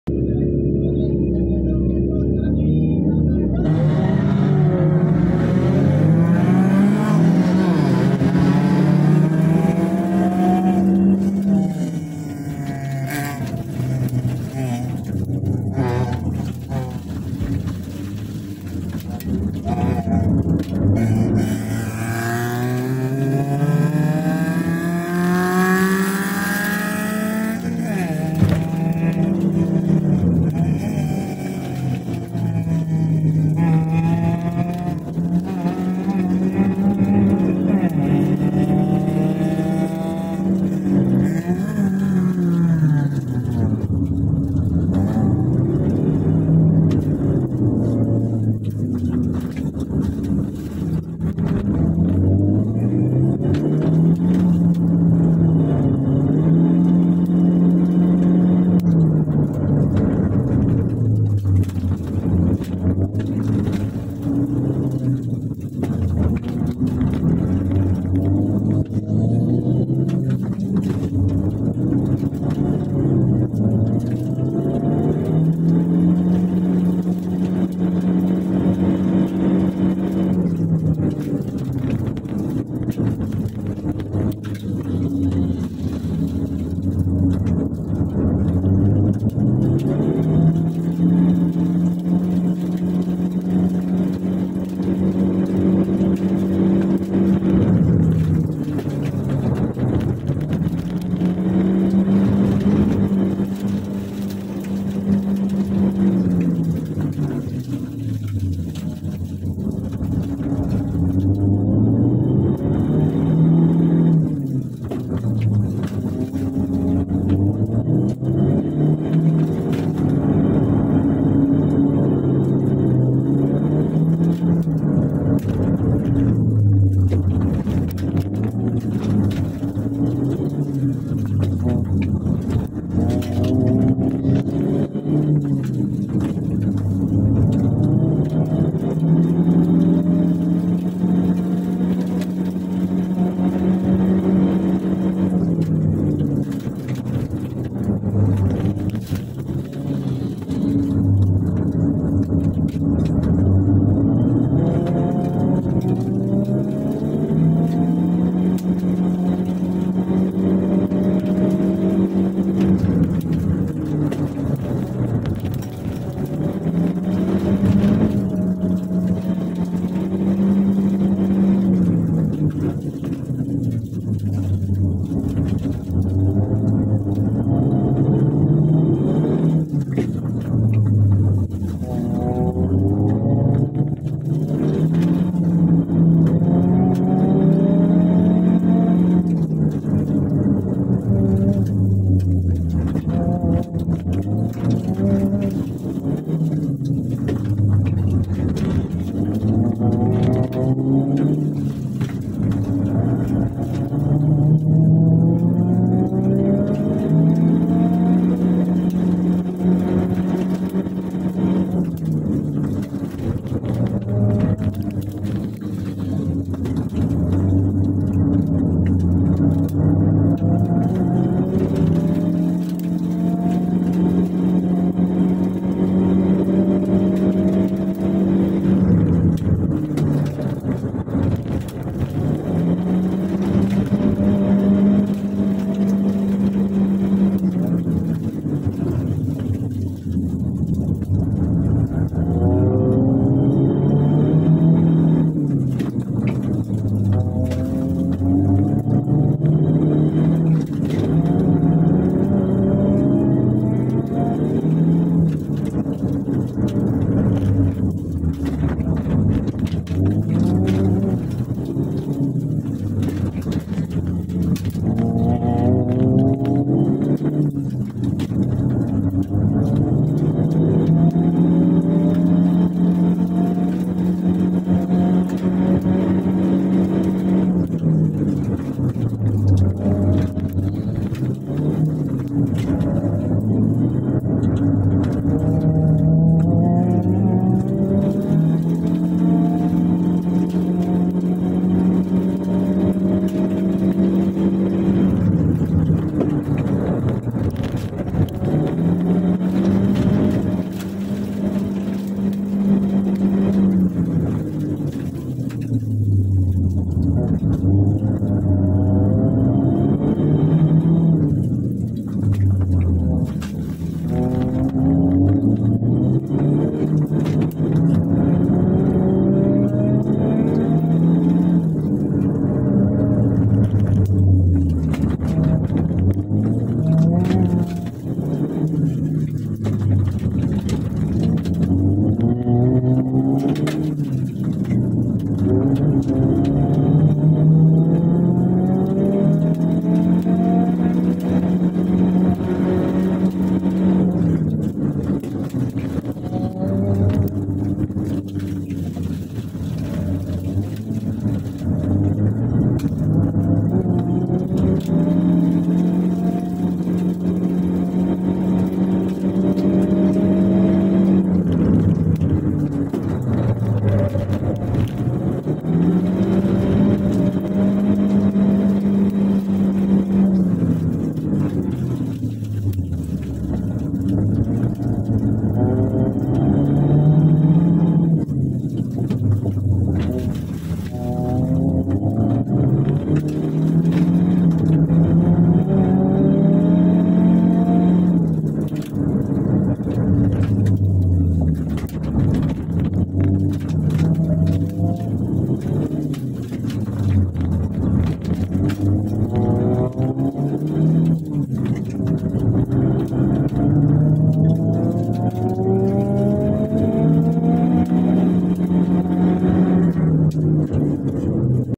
Campro VS Campro •Event• DSSS 2022 R1 06Feb (lawan jam ➡️➡️➡️) onboard Saga FLX CVT
Dato Sagor Super Series 2022 [ Round 1] 06-Feb

Category: Campro

Car onboard : Saga FLX CVT #33